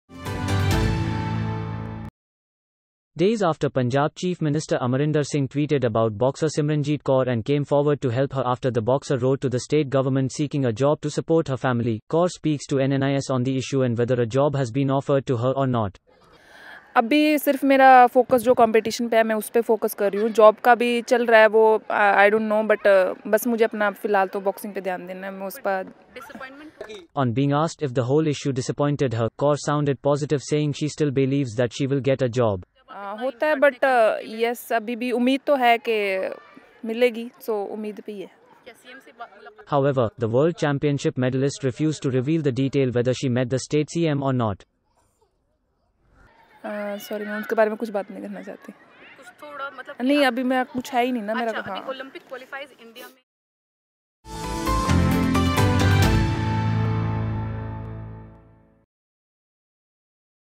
BOXER SIMRANJIT STILL HOPEFUL OF A JOB POST AMARINDER SINGH’S TWEET
Days after Punjab Chief Minister Amarinder Singh tweeted about boxer Simranjit Kaur and came forward to help her after the boxer wrote to the state government seeking a job to support her family, Kaur speaks to NNIS on the issue and whether a job has been offered to her or not.